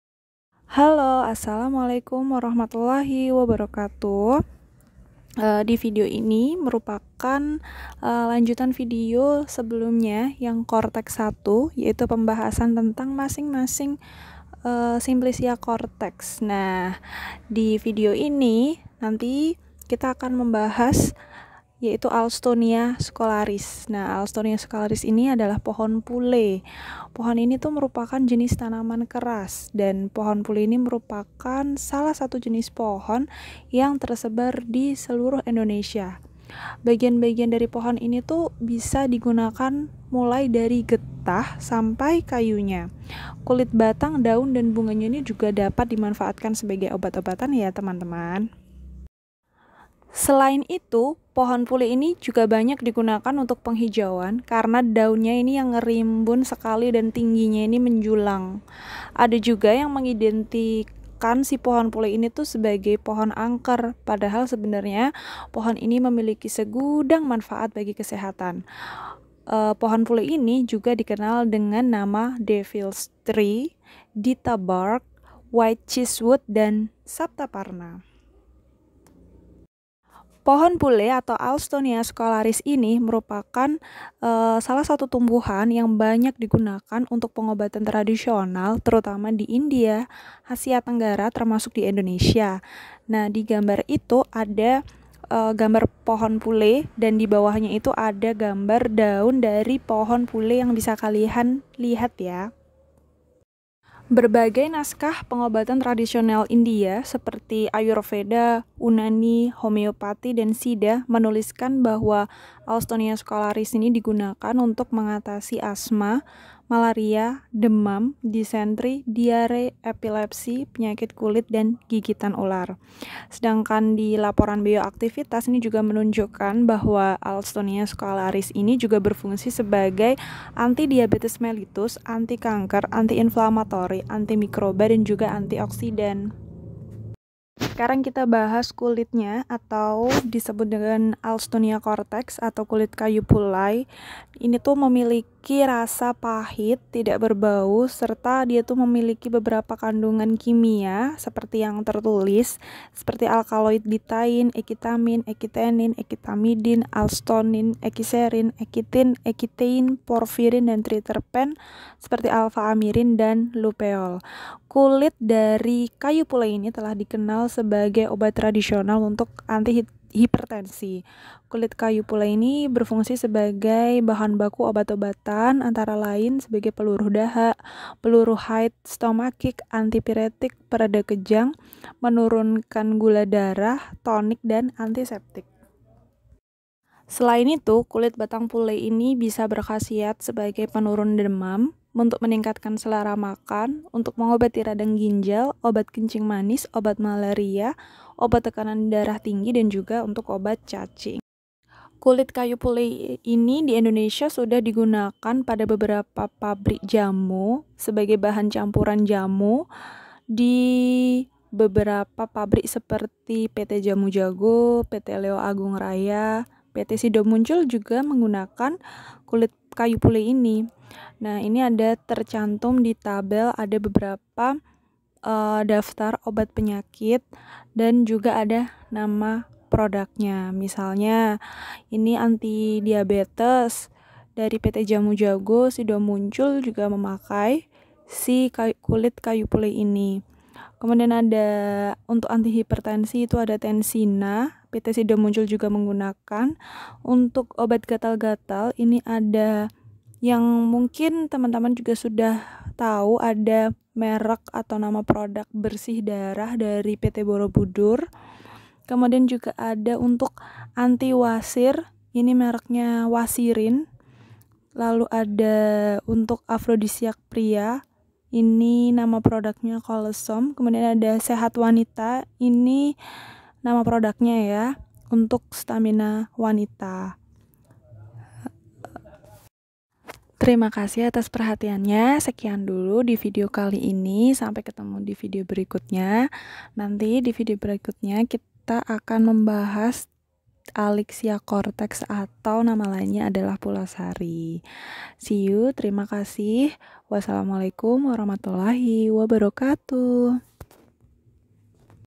Manfaat pohon pule , manfaat kulit kayu pule ,alstonia cortex
Assalamualaikum, di video kali ini saya akan membahas tentang Alstonia cortex atau kulit kayu pule